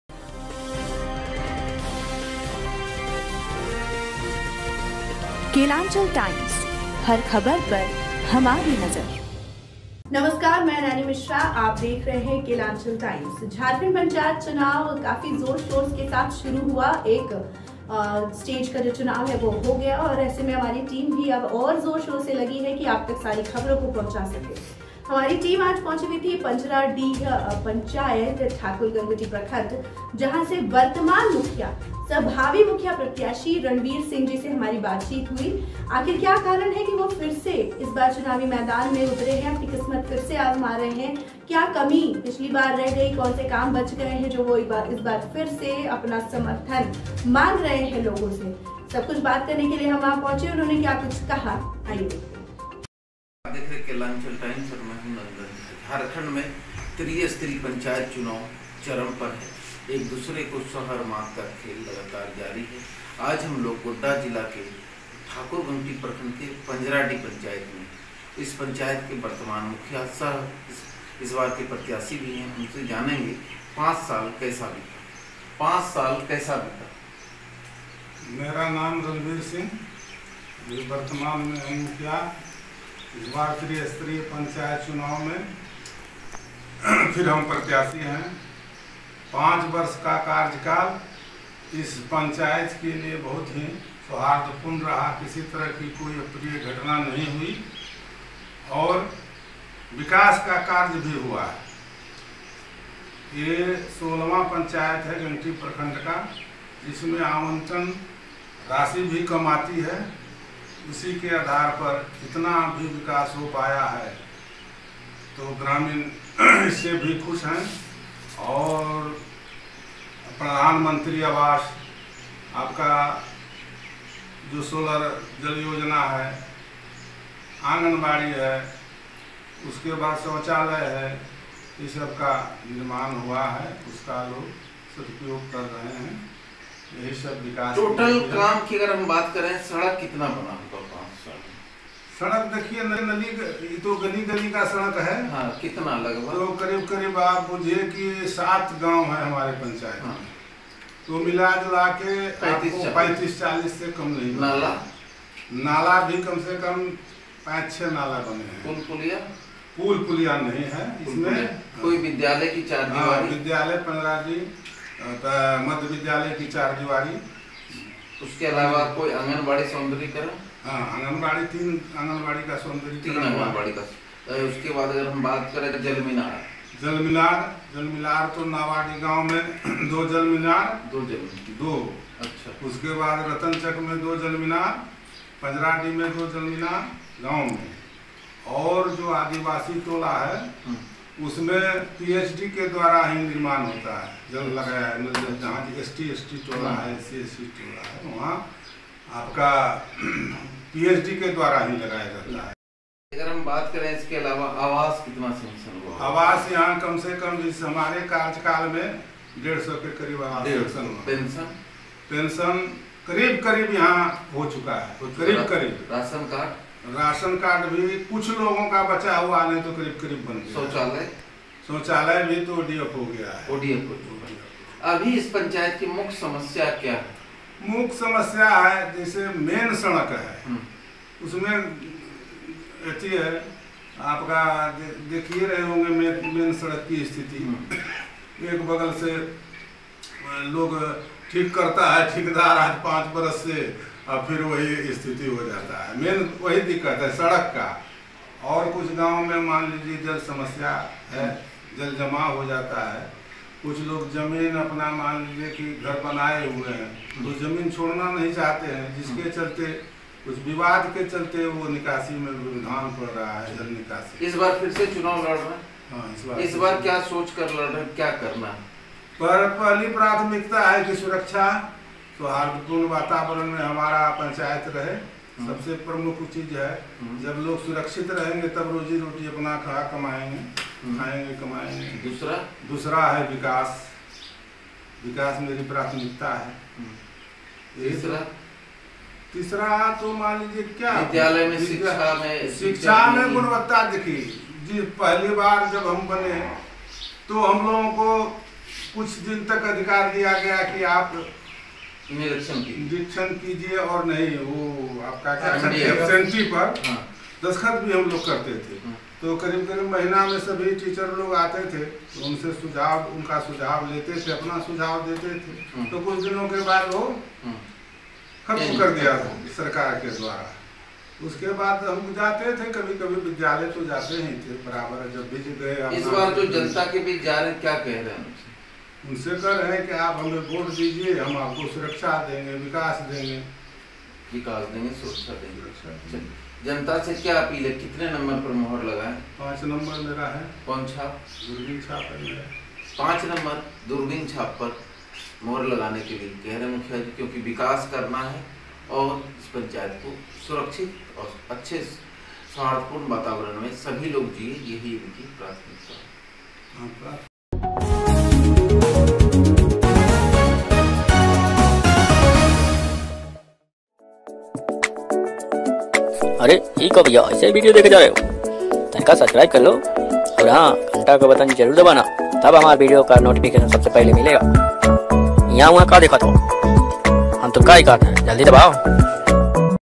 पंचायत पंजराडीह क्रम संख्या 5 से वर्तमान मुखिया सह भावी मुखिया प्रतियाशी रणवीर सिंह से मुलाकात II
प्रखण्ड ठाकुर गंगटी पंचायत पंजराडीह क्रम संख्या 5 से वर्तमान मुखिया सह भावी मुखिया प्रतियाशी रणवीर सिंह से ख़ास मुलाकात II